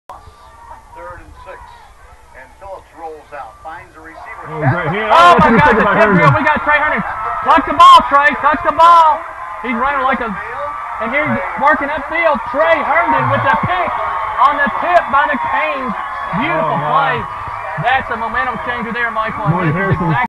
Tre Herndon Interception
EHS Junior intercepts SM QB on 1st possession of the game.